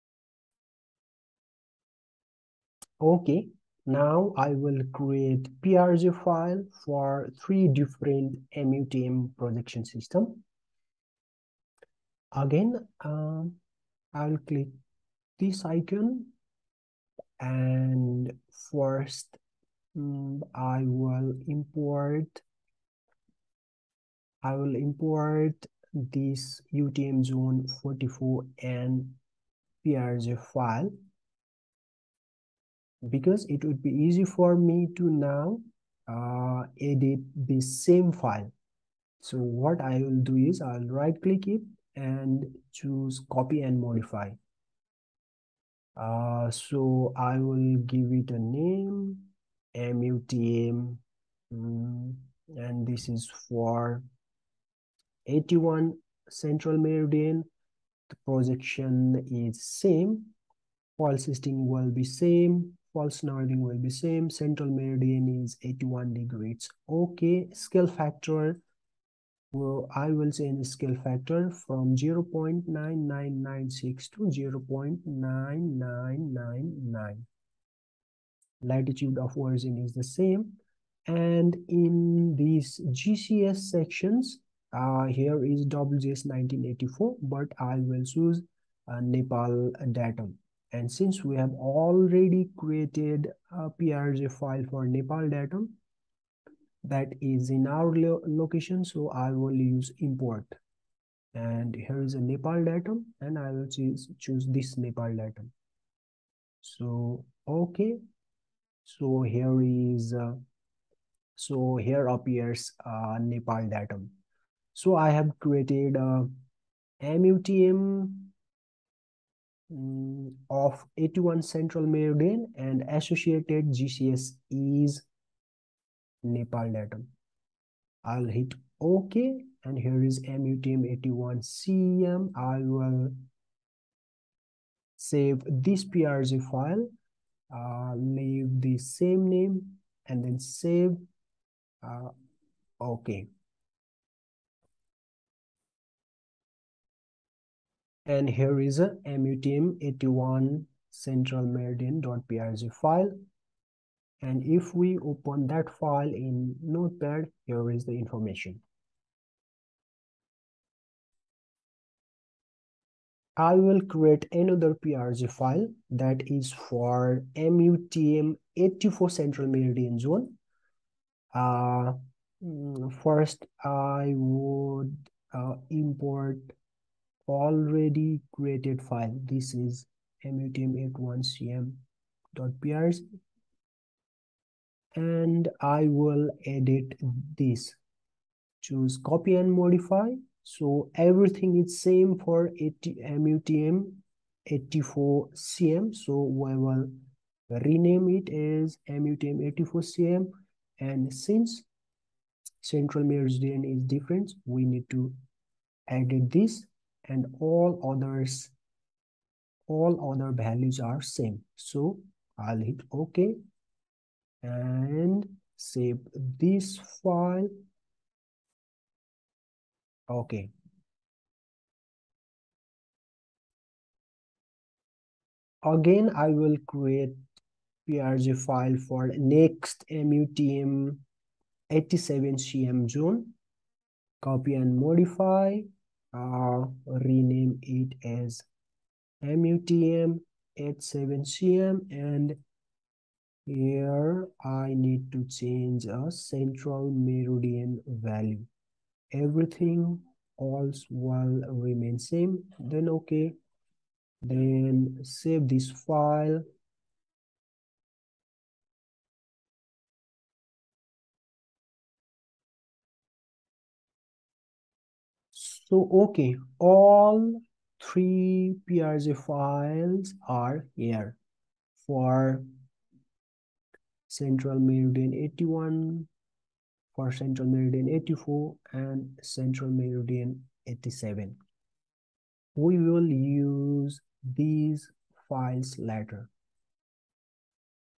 Creating Projection File (.prj) for MUTM 81CM, MUTM 84CM & MUTM 87CM Zone in ArcMap - 11/18
One of the constituent file of shapefile format is .prj file.  This file stores information about coordinates system both geographic coordinate system and projected coordinate system. In this video, .prj file is created for 3 MUTM zones used in Nepal.